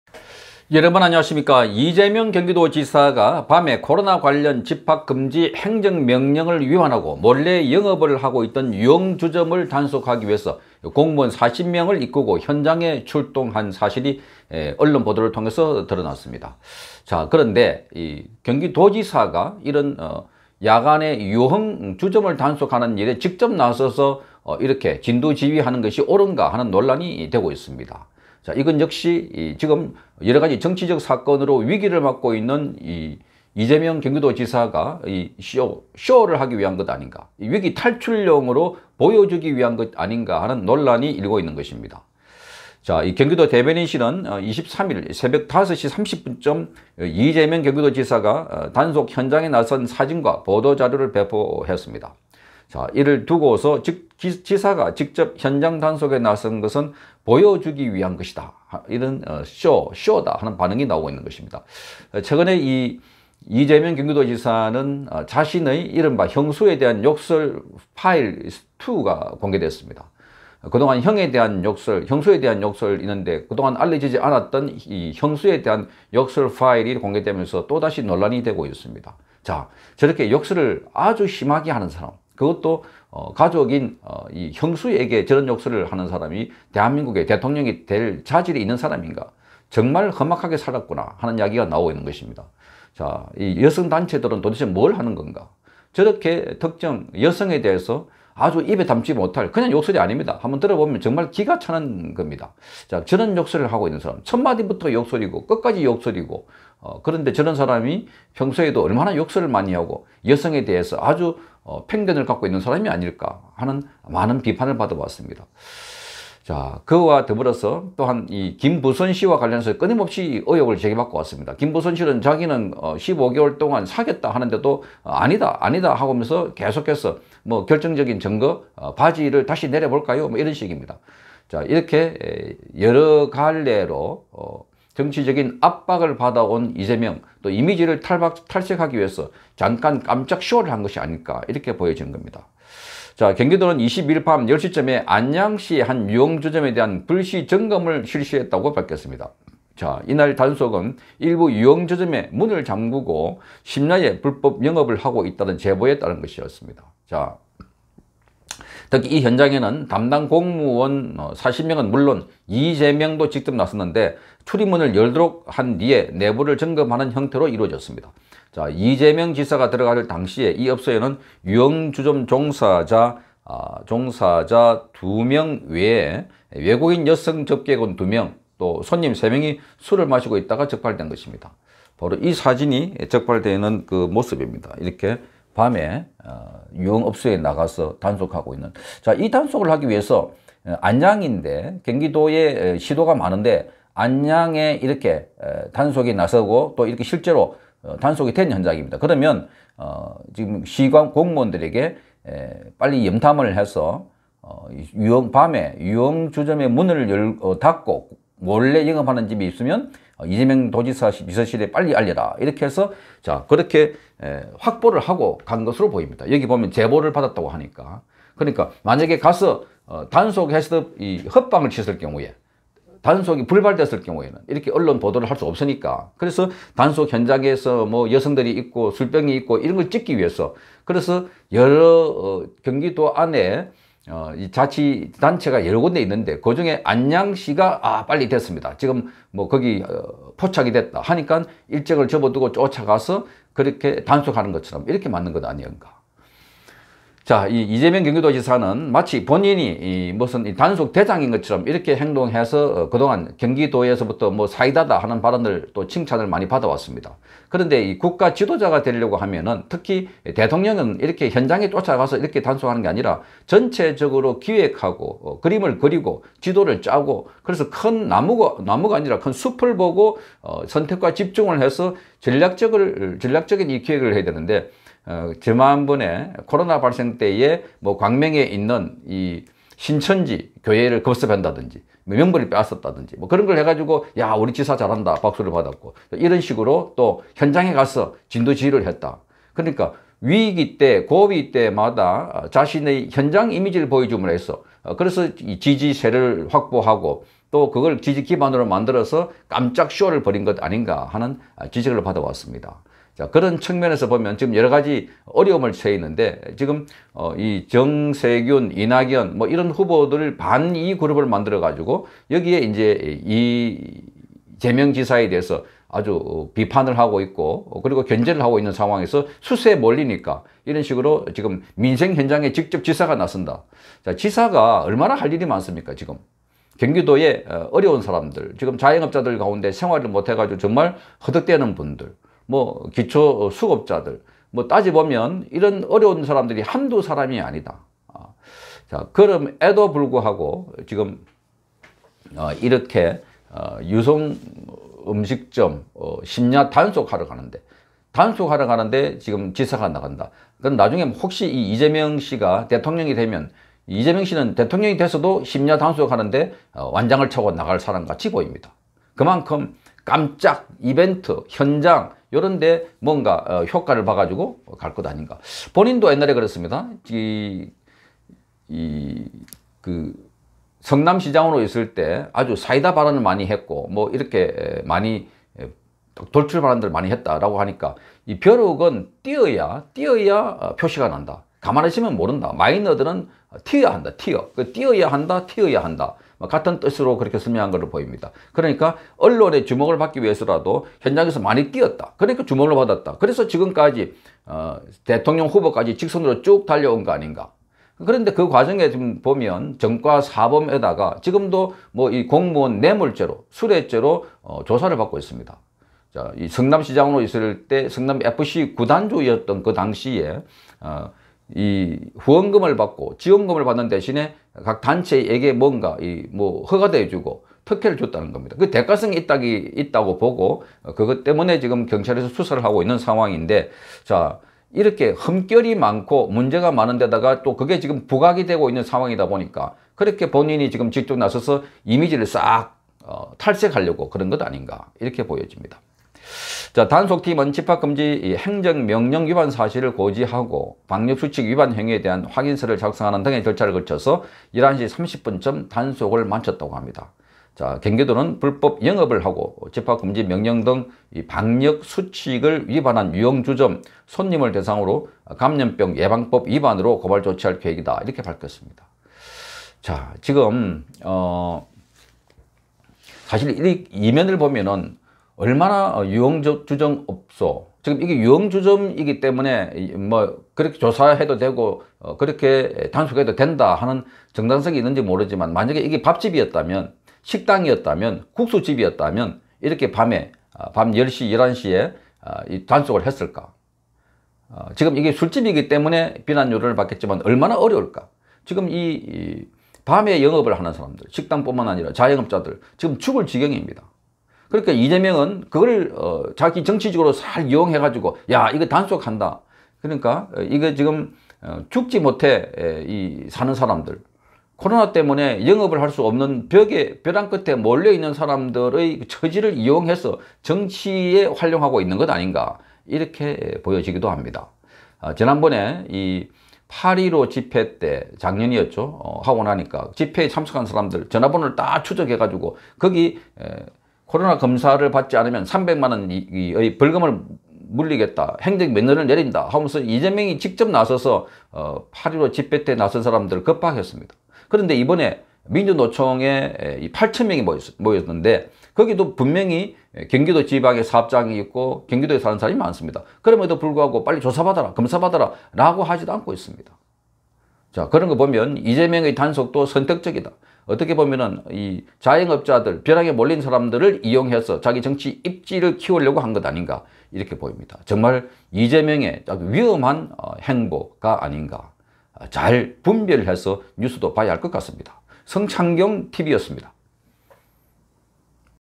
공무원 40명 이끌고… 한밤 유흥주점 단속 나간 이재명. 형수 욕설 파일,김부선 스캔들 덮으려고?
이재명이 유흥업소 단속에 나선 것은 자신의 위기를 모면하려는 정치적인 쇼라는 지적